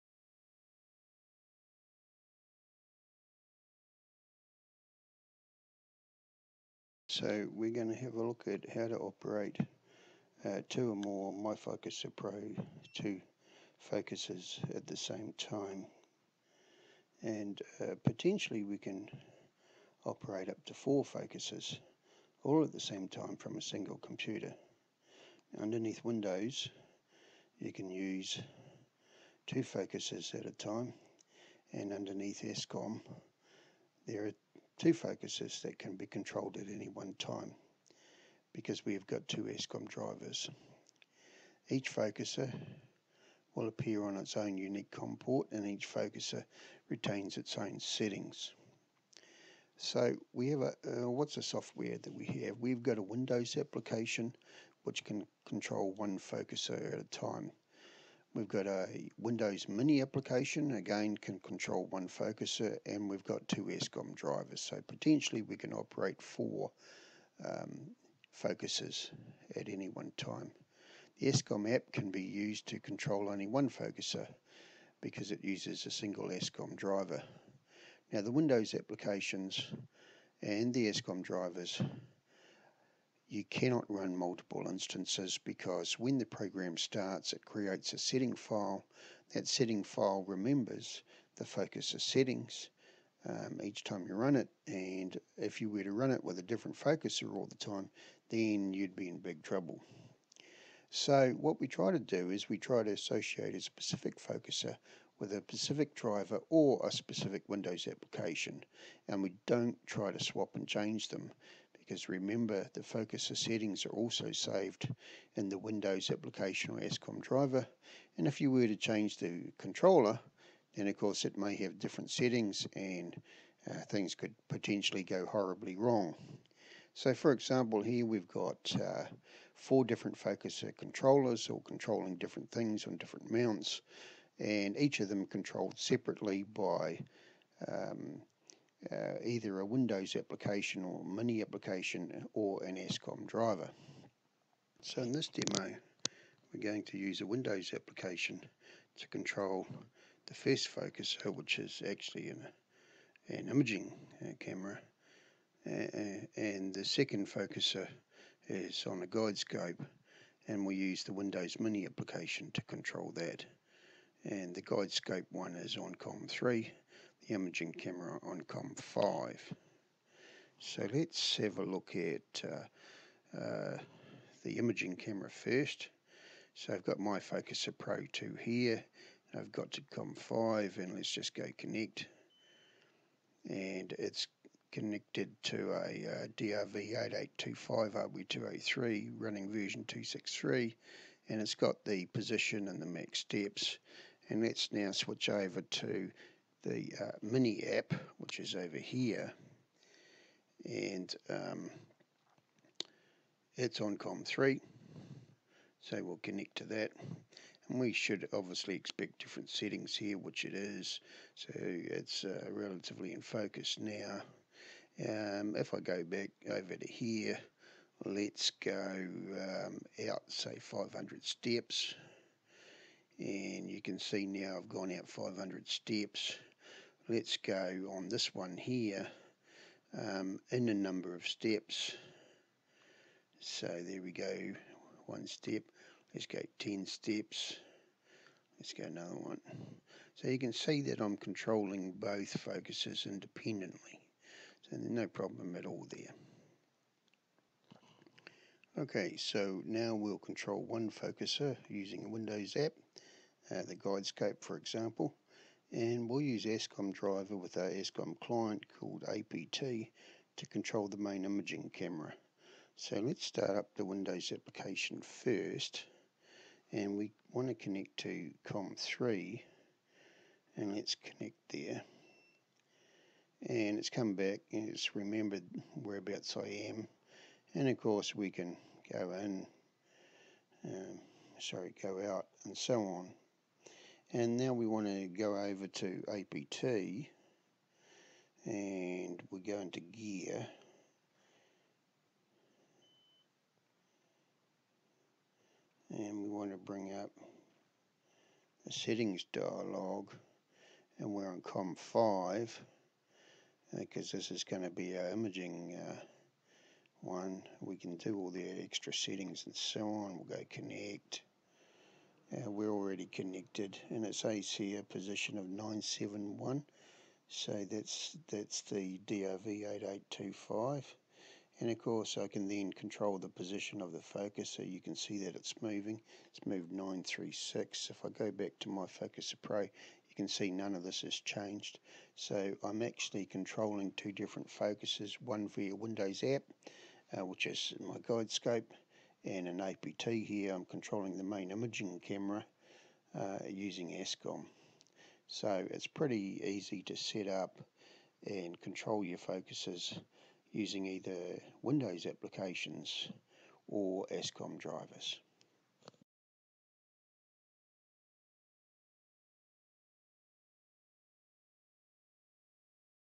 Part3f Controlling more than one myFocuserPro2 controller
myFocuserPro2: How to control more than one myFocuserPro2 focus controller at the same time.